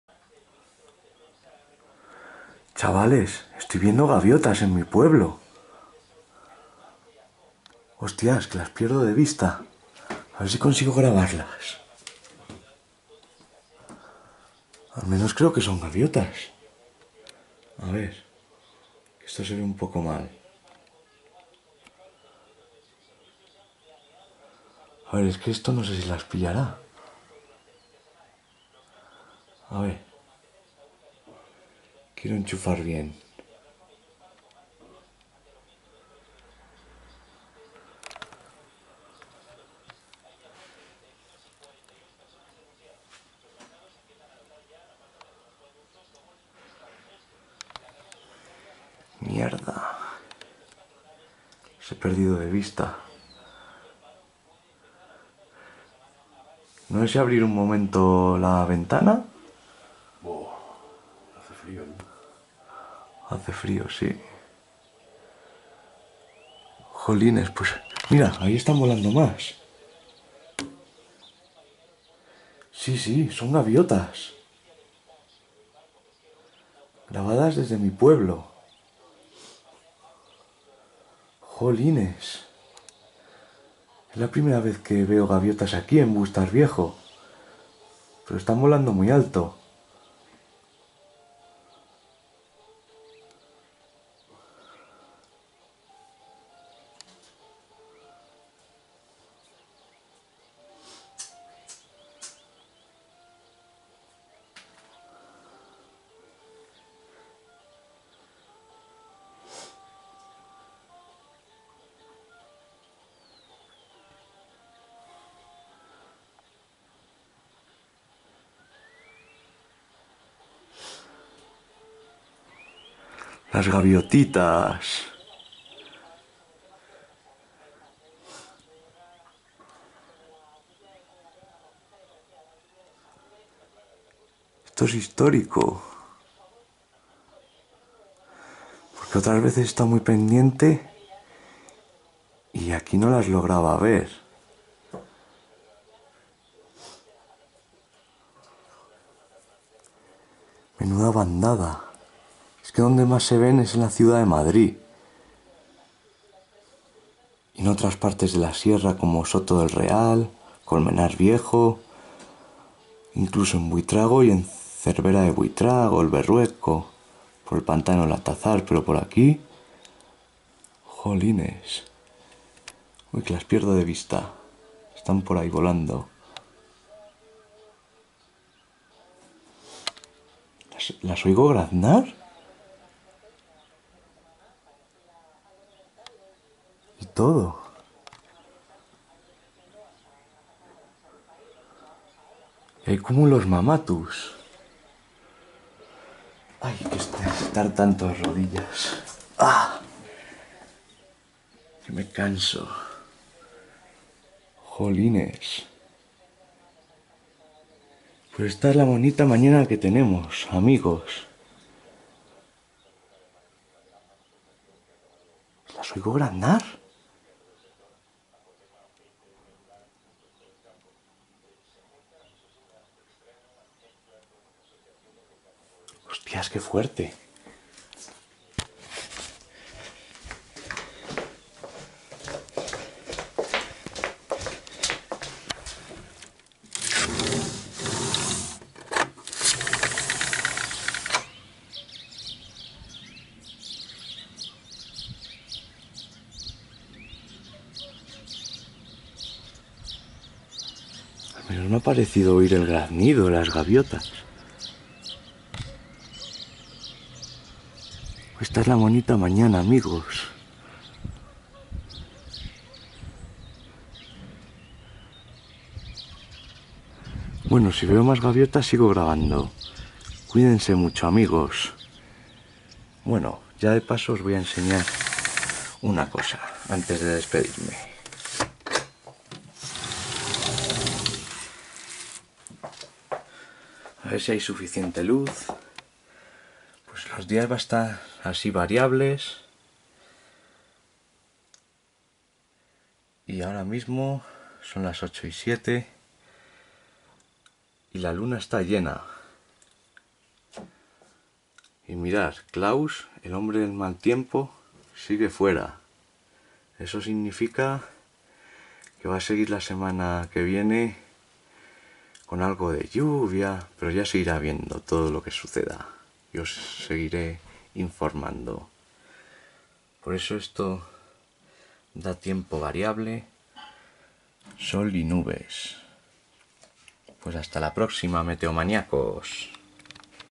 Gaviotas sobrevolando la Sierra de Guadarrama, Bustarviejo, Madrid, España 19 Marzo 2022.
Sierra Norte de Madrid, mañana de cúmulos mammatus con las gaviotas en el cielo. Es normal ver gaviotas en el mar e incluso en la ciudad de Madrid, Colmenar Viejo, Soto del Real, en el embalse de Santillana de Manzanares El Real, en el embalse del Atazar, Buitrago e incluso Somosierra que también es difícil verlas allí pero en Bustarviejo Más porque estamos casi rodeados de monte y solo se ve el horizonte mirando hacia el este y si estas por la mina de la Plata y valle de los Abedules mirando en dirección suroeste que a veces si el horizonte esta limpio se ven los montes de Toledo y por la noche la silueta de los focos de la contaminación lumínica de Alcorcón y Móstoles. Esto esta grabado en el día del padre en el 19 de Marzo de 2022. Si estás atentos a partir del minuto 2:53 oiréis el graznido de las gaviotas y quién tenga buen oído.